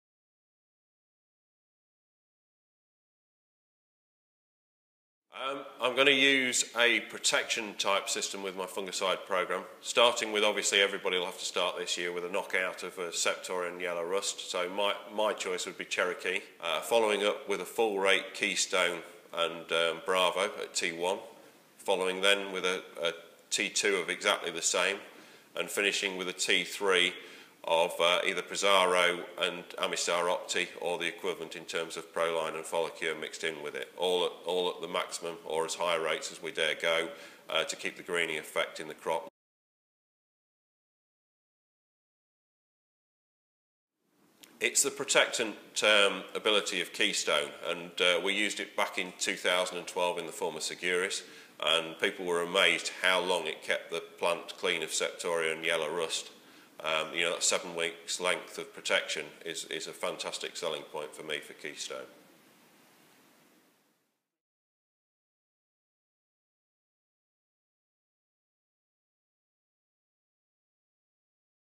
Fungicides for a record yielding wheat crop
Record breaking grower Tim Lamyman tells us about how he uses a preventative approach to maximise yields on his farm.